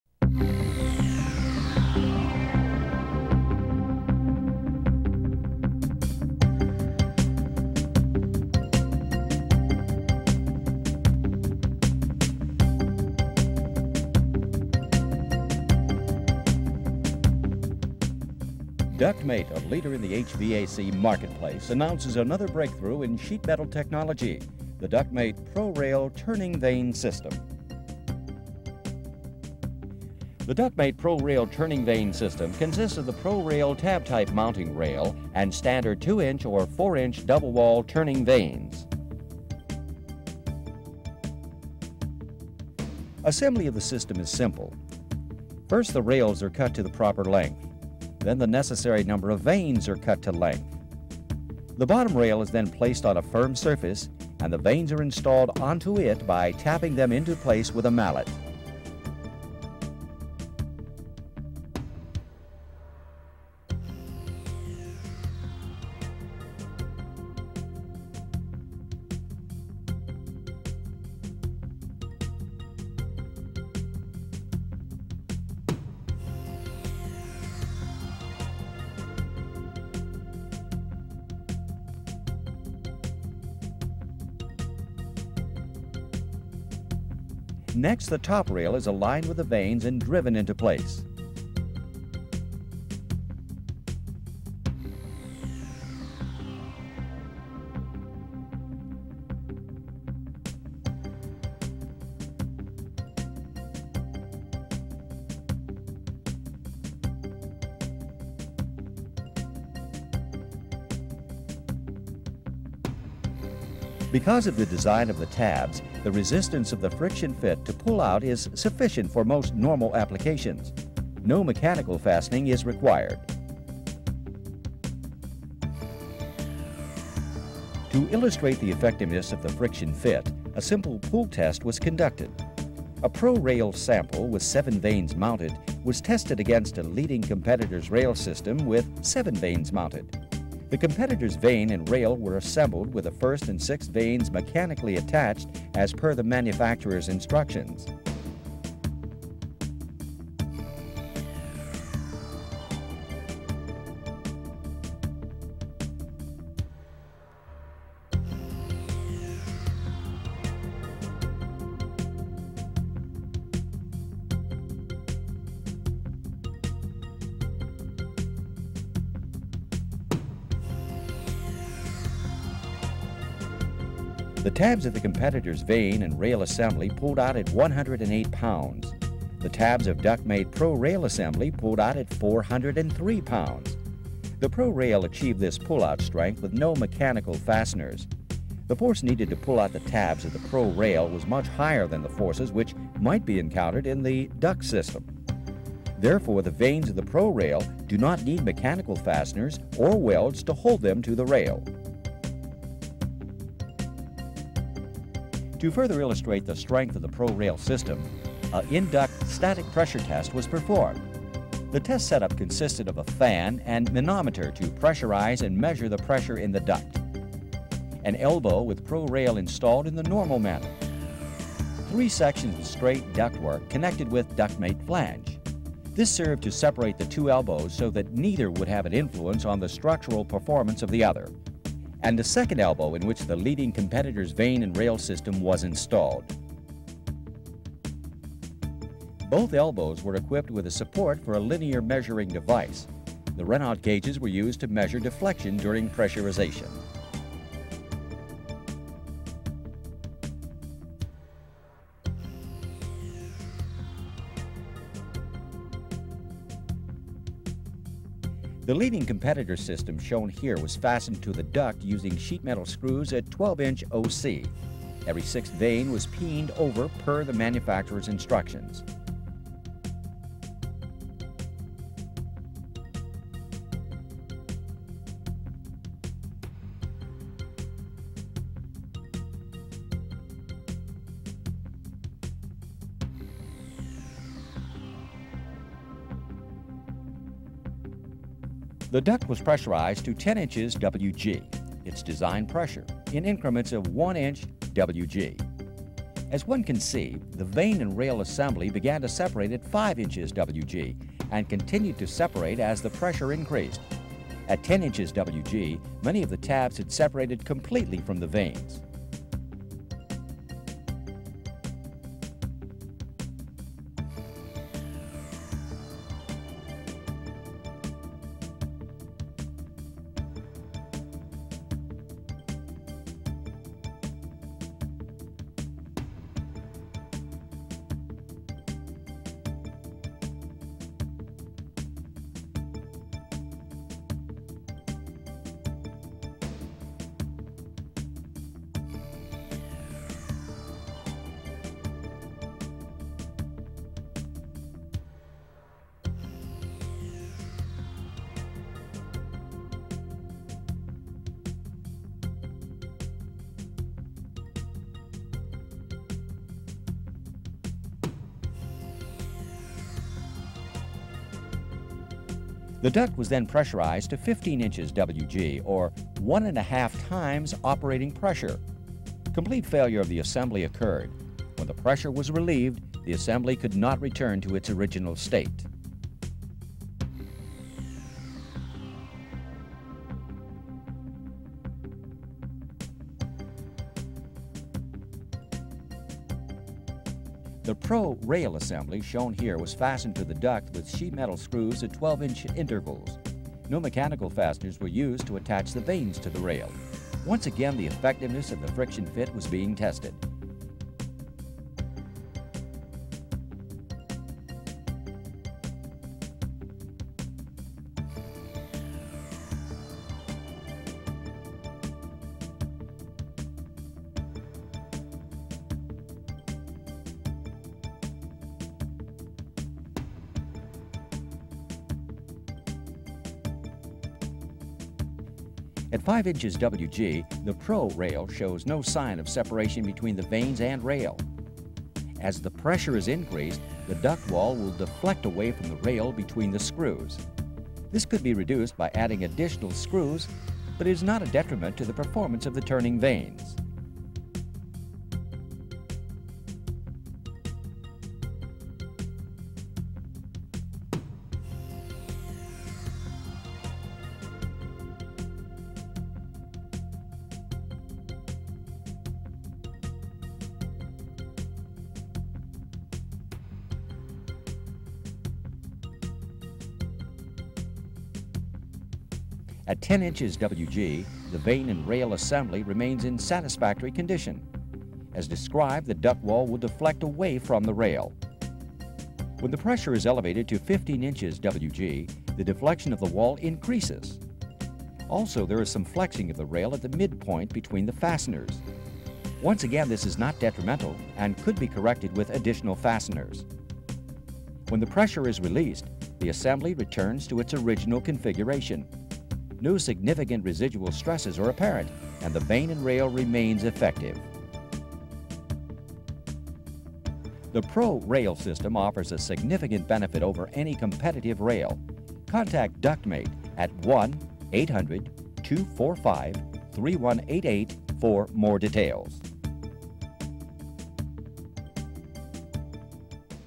Ductmate Industries Inc. PROrail Instructional Video
PROrail provides a self-aligning, easy means of securing turning vanes for the purpose of directing air around mitered elbows.